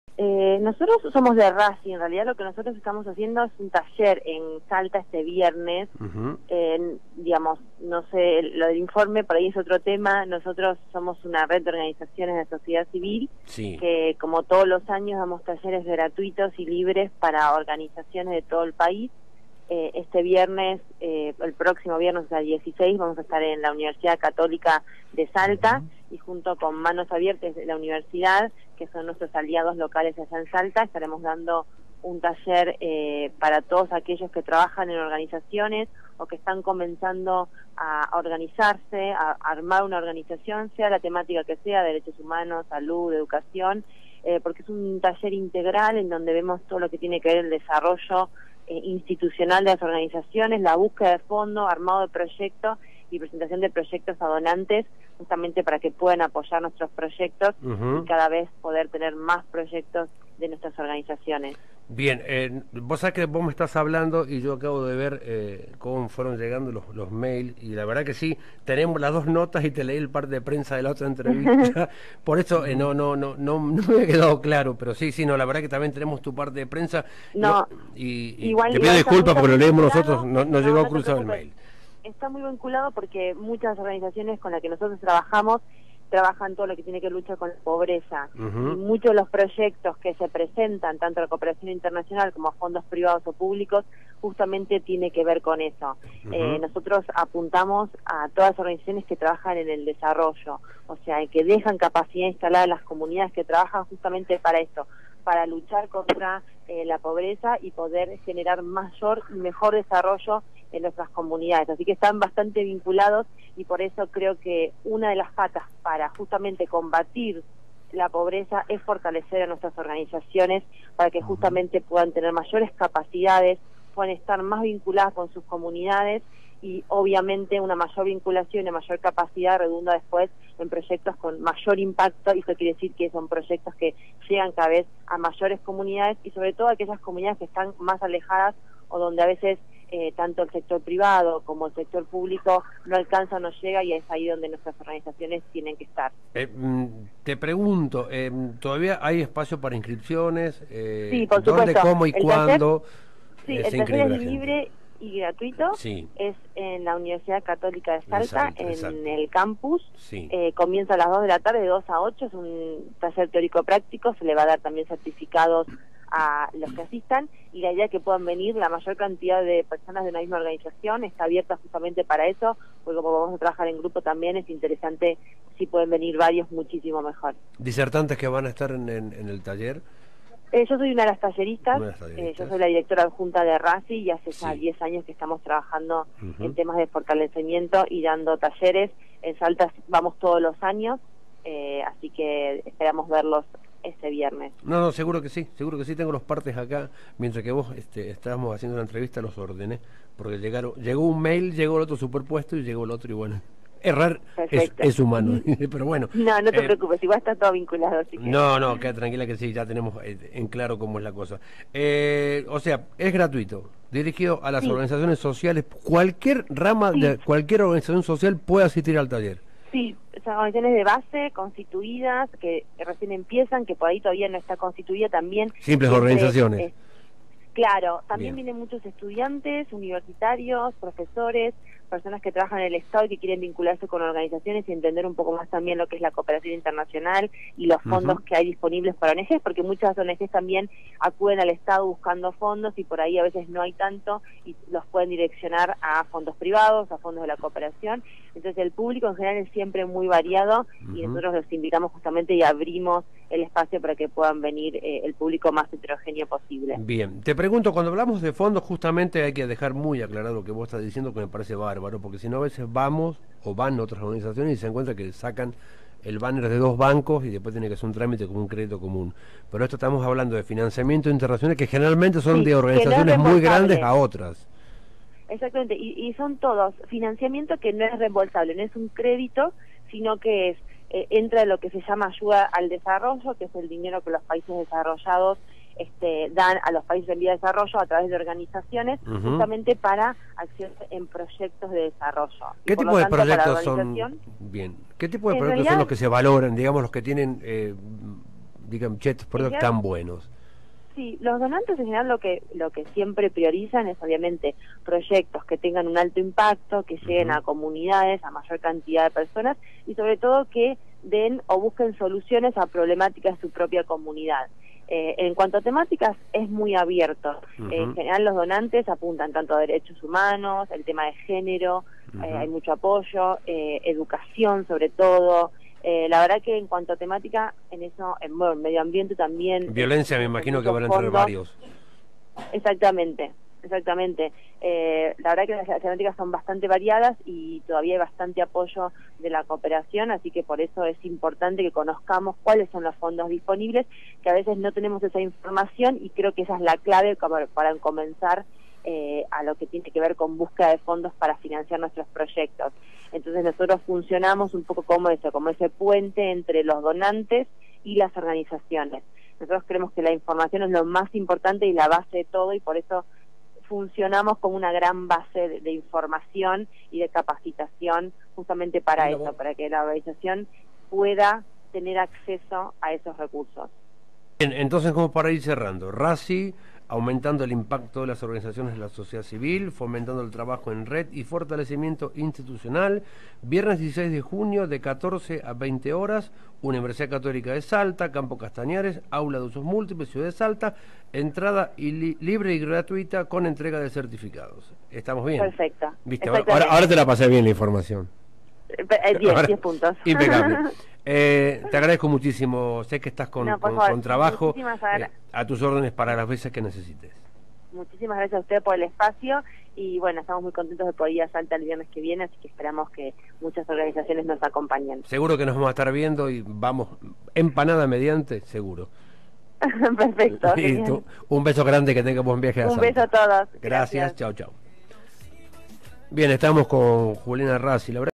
JULIANA RACI: Capacitación Aumentando el impacto de las Organizaciones de la Sociedad Civil
COMUNICACIÓN TELEFÓNICA CON JULIANA RACI:
RACI llevará a cabo la jornada de capacitación “Aumentando el impacto de las Organizaciones de la Sociedad Civil: Fomentando el trabajo en Red y el fortalecimiento institucional”.
El taller está destinado a instituciones que deseen adquirir nuevas herramientas e información para potenciar el desarrollo institucional al interior de sus Organizaciones.
La jornada es totalmente gratuita y se realizará el viernes 16 de junio de 2017 de 14.00 hs a 20.00 hs, en la Universidad Católica de Salta, Campo Castañares (aula 5 UM2).
Los objetivos del encuentro son fortalecer a las OSC - en especial a las organizaciones de base - para que puedan acceder en mejores condiciones institucionales y técnicas a los inversores sociales estratégicos orientados al desarrollo a través de jornadas de capacitación in situ; y profundizar la cobertura nacional de las actividades de fortalecimiento institucional, para aportar a la democratización del acceso a la información y formación para las OSC del interior del país.